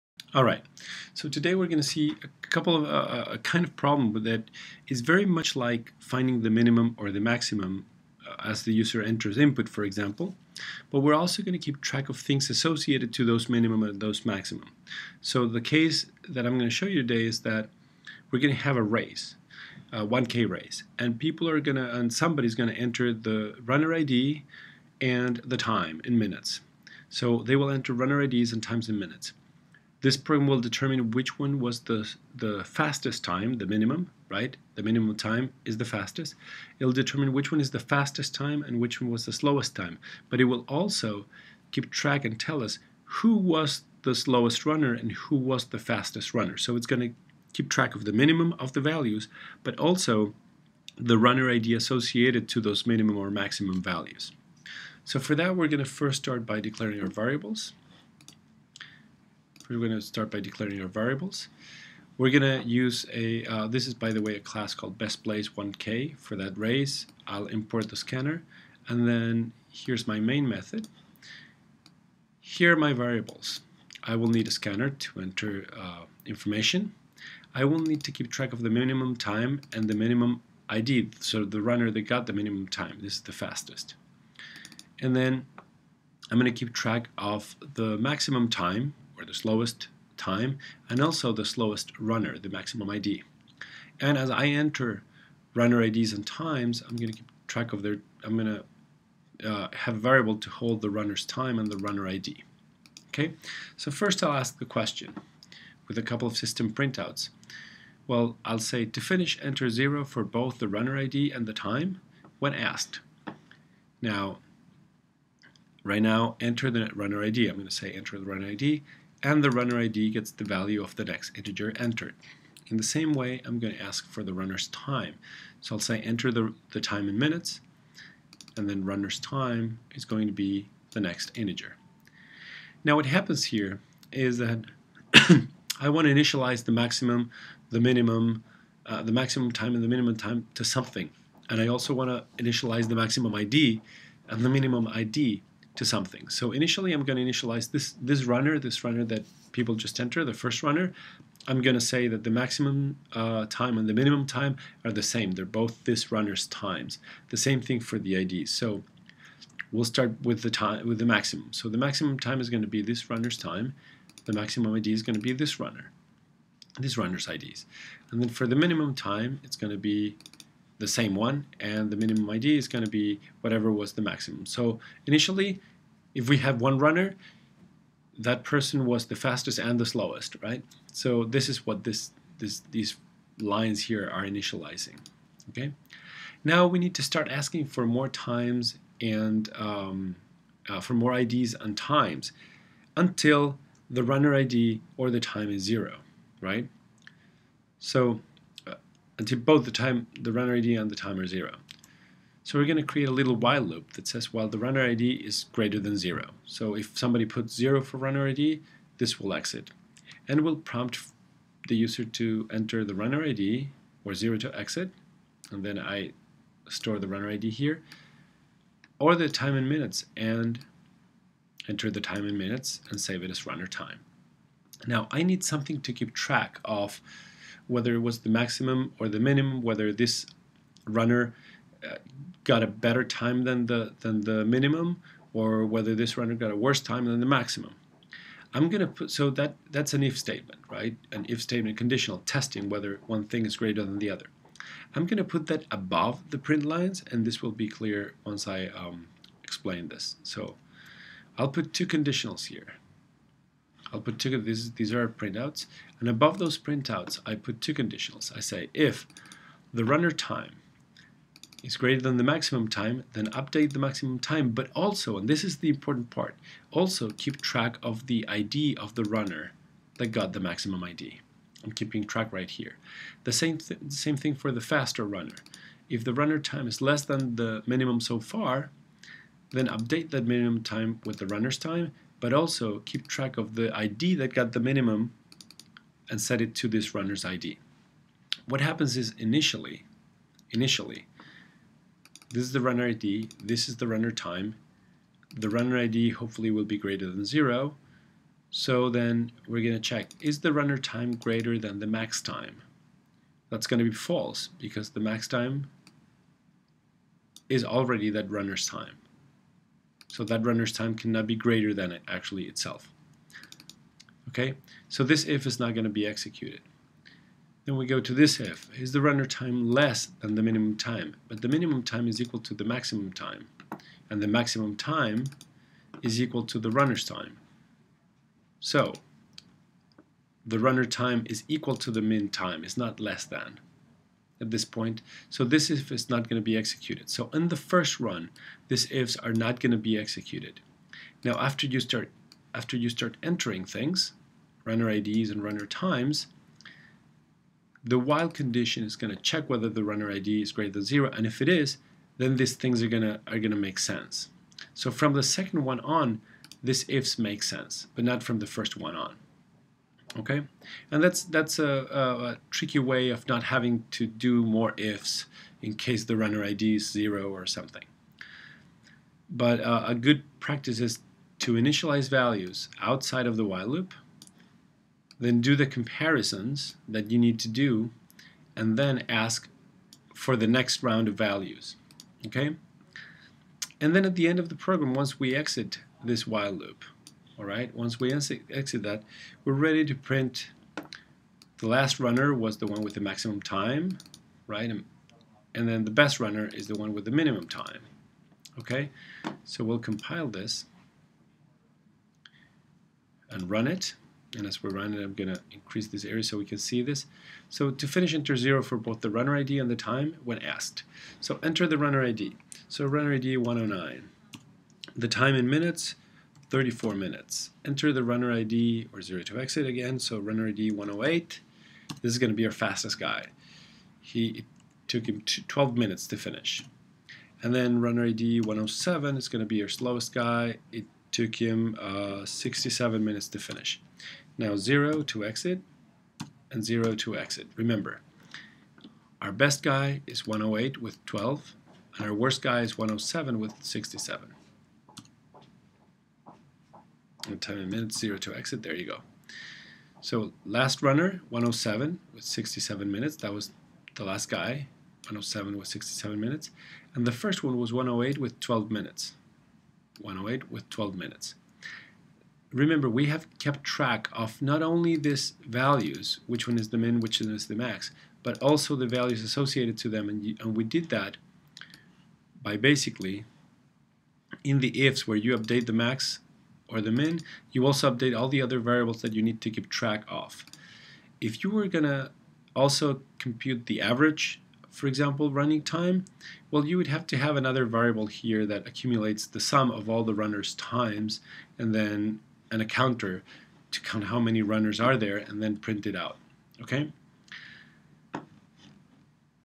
argmax argmin
This video shows how to determine the minimum and maximum and also values associated to them (argmin and argmax as they are called). For example, the following program shows how users can enter runner numbers and their times and then the program tells them not only which was the best and worst times, but also which runners got the best and worst times.